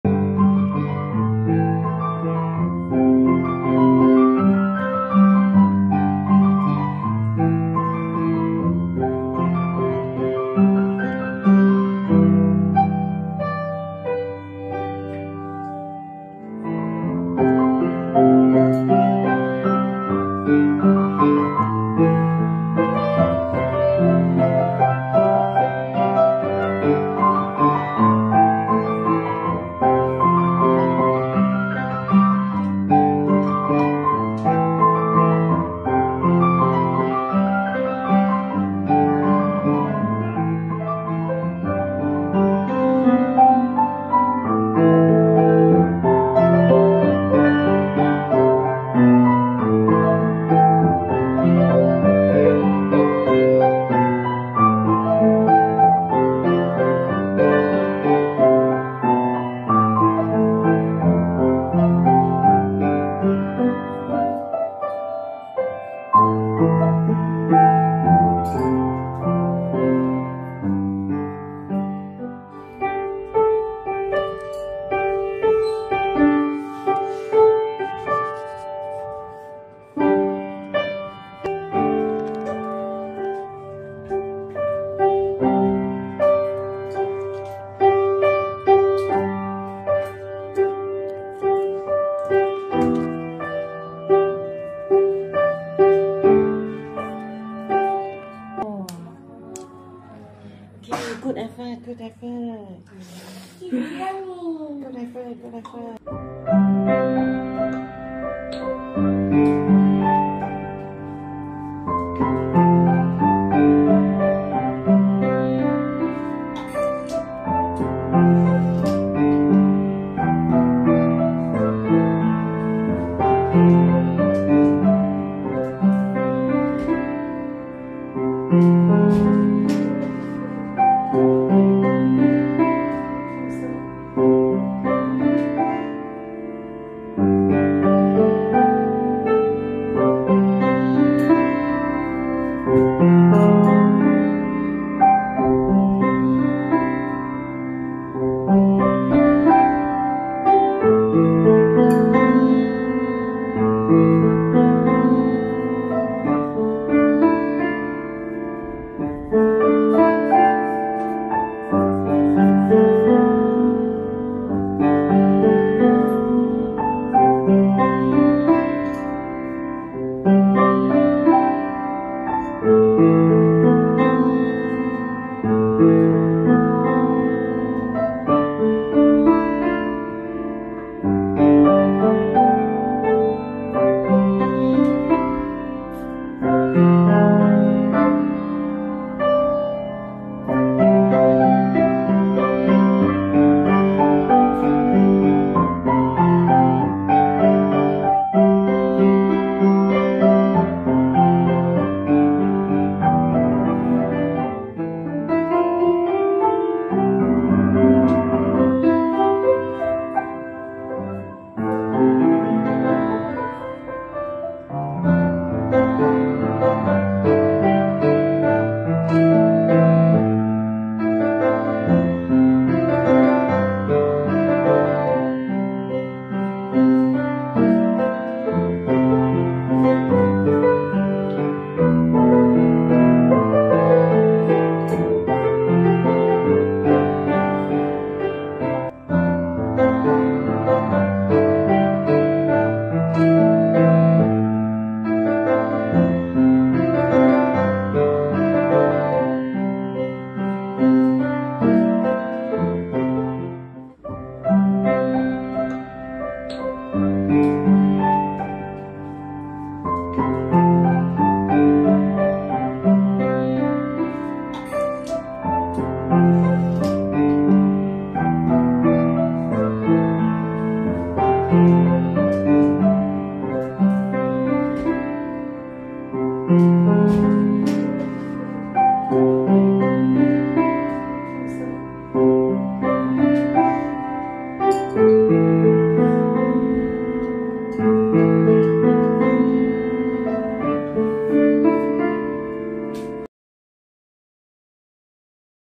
Live Modeling: sketch with friends
Training sketching skills  in speed, observation, focus, decision, lines, colors, elements, fun and many more.